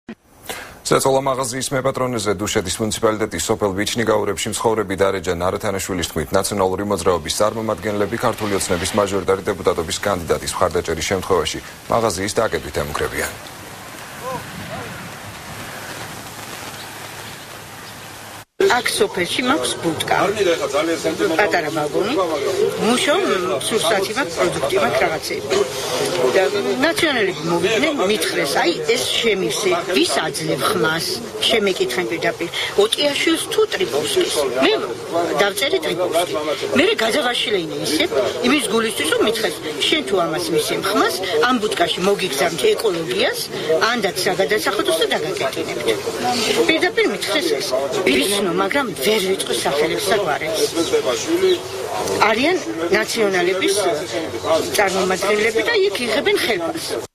ახალი 3 | ზეწოლა მაღაზიის მეპატრ. | 24.08.12
ზეწოლა მაღაზიის მეპატრონეზე. დუშეთის მუნიციპალიტეტის სოფელ ბიჩნიგაურებში მცხოვრები დარეჯან არათანაშვილის თქმით, ნაციონალური მოძრაობის წარმომადგენლები ქართული ოცნების მაჟორიტარი დეპუტატობის კანდიდატის მხარდაჭერის შემთხვევაში, მაღაზიის დაკეტვით ემუქრებიან.